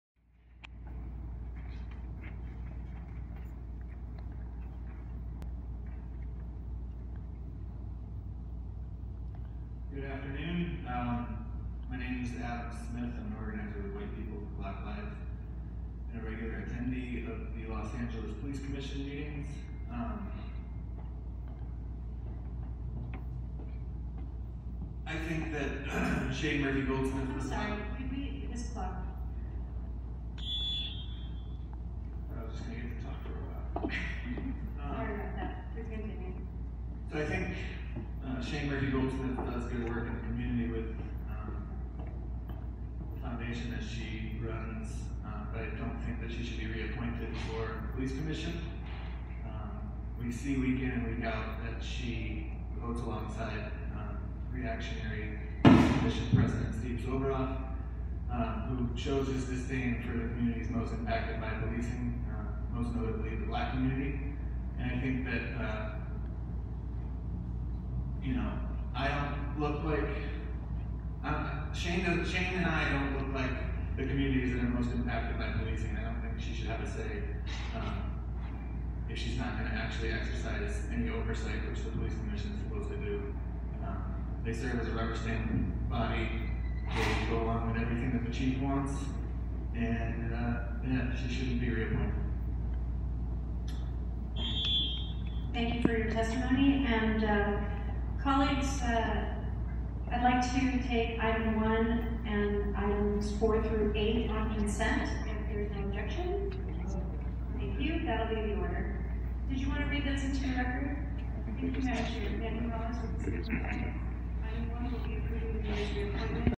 2019 05 22 la city council public safety committee adam smith comment on shane goldsmith reappointme
This is a comment by White People for Black Lives activist Adam Smith opposing the reappointment to the Los Angeles Police Commission of Shane Goldsmith.